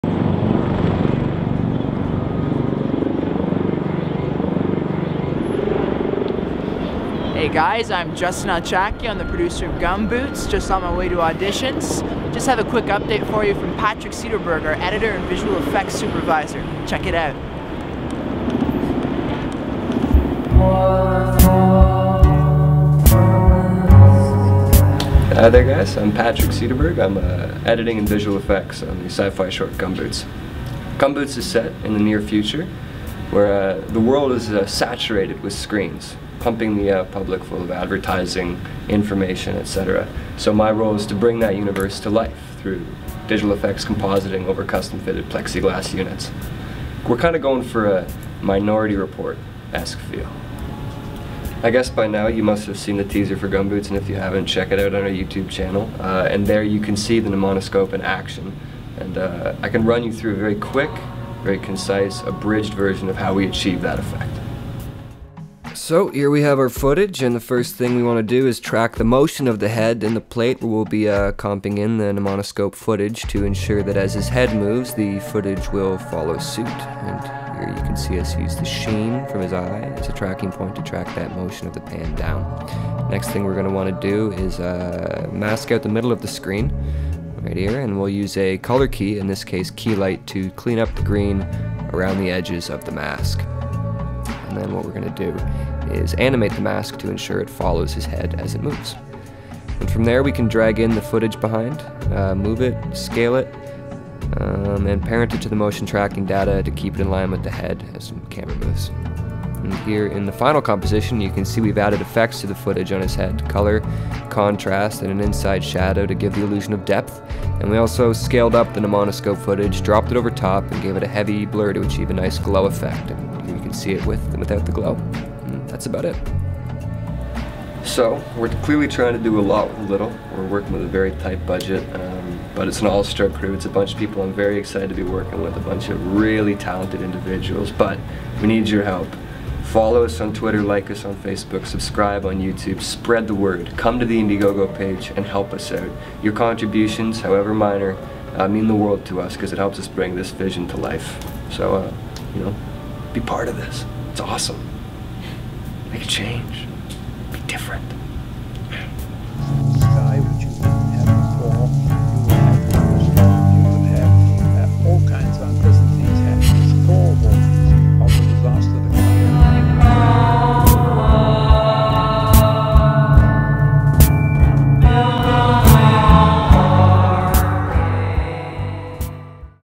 Gumboots - In The Edit Suites
Meet our explosively talented Editor and VFX Coordinator Patrick Cederberg as he demonstrates the method behind our madness in this quick little after effects tutorial.